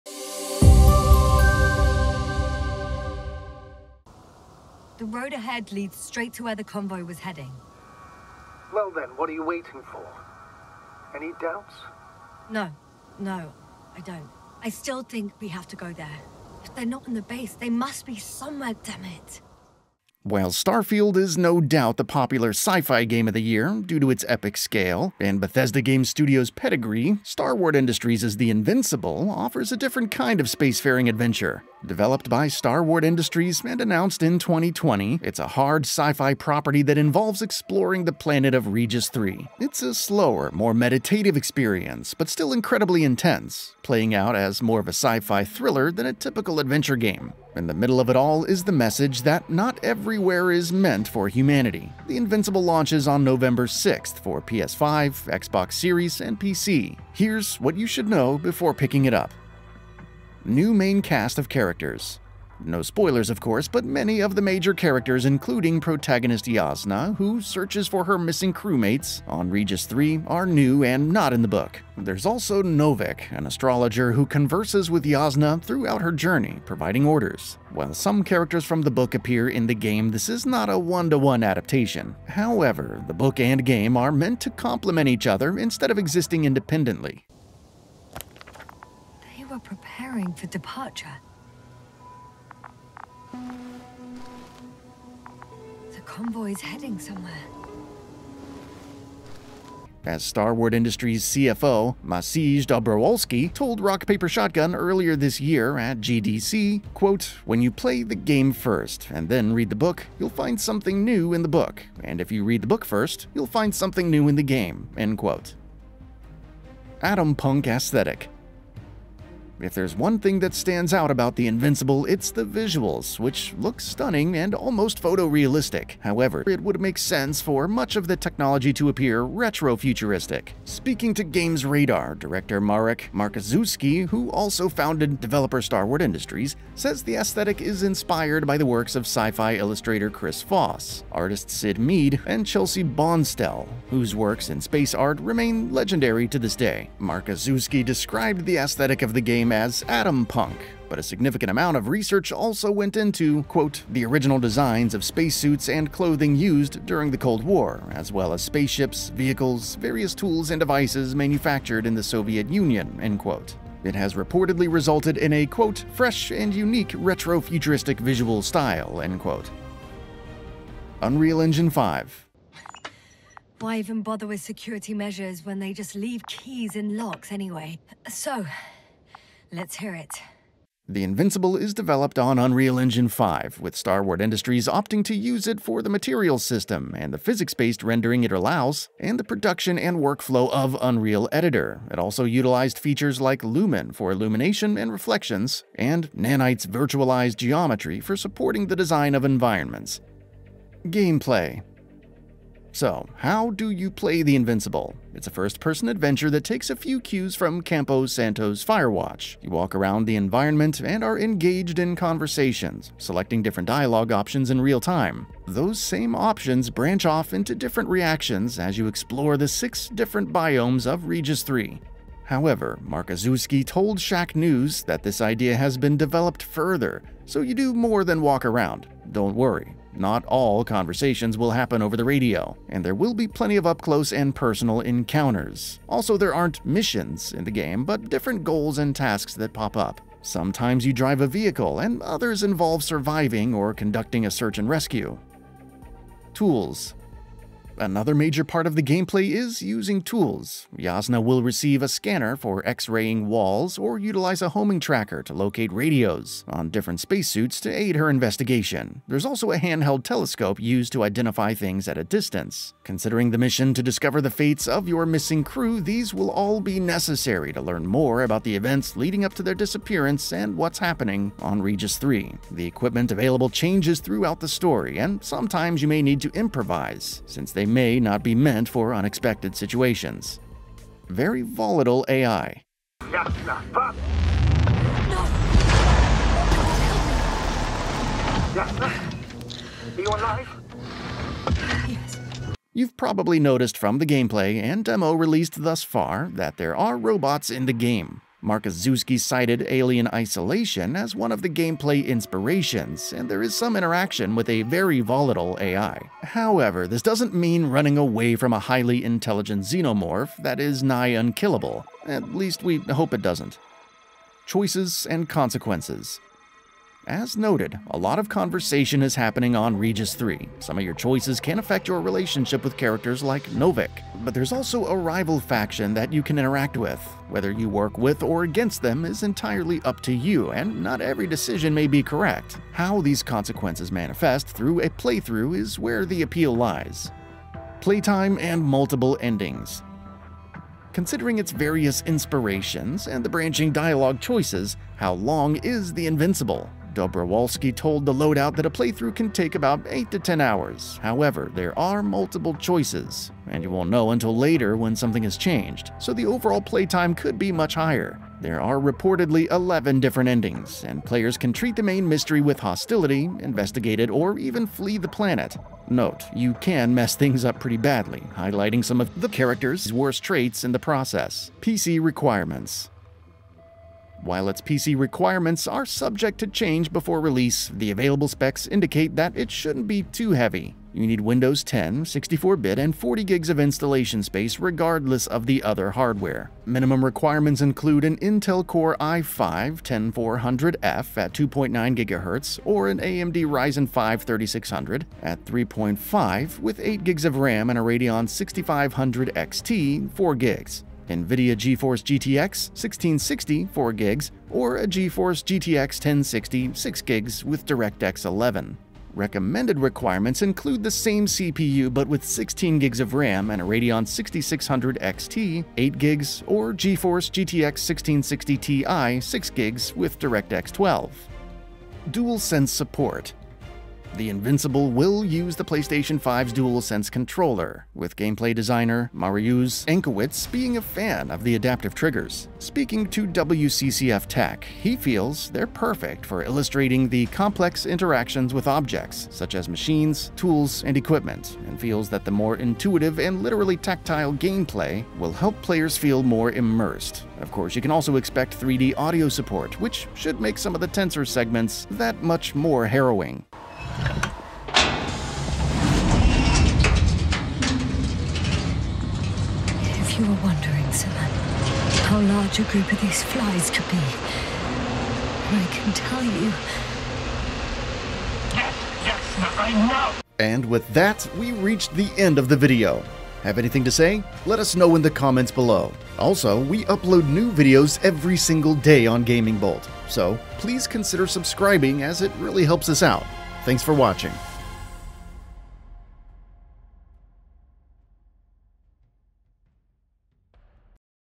The Invincible - 10 Things You ABSOLUTELY NEED To Know Before You Buy
Announced in 2020, Starward Industries' The Invincible launches on November 6th for Xbox Series X/S, PS5 and PC. The game sees astrobiologist Yasna venturing to Regis 3 and searching for her lost crewmates.

What starts as an investigation quickly evolves into a battle for survival and coming to terms with the planet's hostility. With first-person gameplay, a dialogue system akin to Firewatch, and branching choices leading to multiple endings, this sci-fi thriller has a lot to unpack.

Here's everything you should know about The Invincible before it launches, from its atompunk aesthetic and use of Unreal Engine 5 to how long a typical playthrough can take and what you'll be doing all the while.